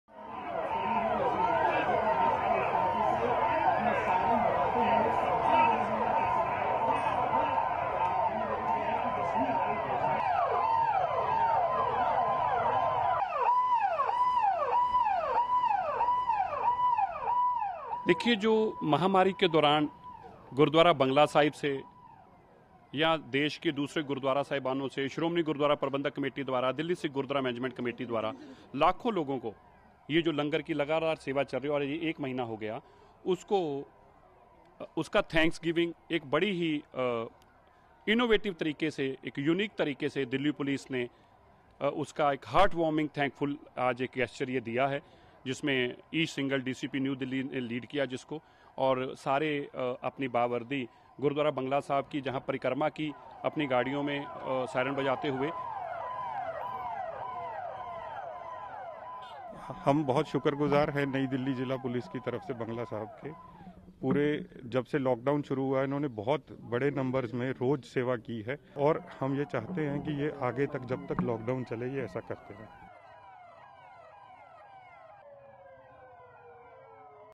ਦਿੱਲੀ ਪੁਲਿਸ ਨੇ ਕੀਤੀ ਗੁਰਦੁਆਰਾ ਬੰਗਲਾ ਸਾਹਿਬ ਦੀ ਪਰਿਕਰਮਾ | BBC NEWS PUNJABI
ਦਿੱਲੀ ਵਿੱਚ ਪੁਲਿਸ ਨੇ ਧੰਨਵਾਦ ਵਜੋਂ ਗੁਰਦੁਆਰਾ ਬੰਗਲਾ ਸਾਹਿਬ ਦੀ ਪਰਿਕਰਮਾ ਕੀਤੀ। ਲੰਗਰ ਸੇਵਾ ਲਈ ਖ਼ਾਸ ਤੌਰ ਤੇ ਧੰਨਵਾਦ ਕੀਤਾ ਅਤੇ ਜਾਰੀ ਰੱਖਣ ਦੀ ਦਰਖ਼ਾਸਤ ਵੀ ਕੀਤੀ।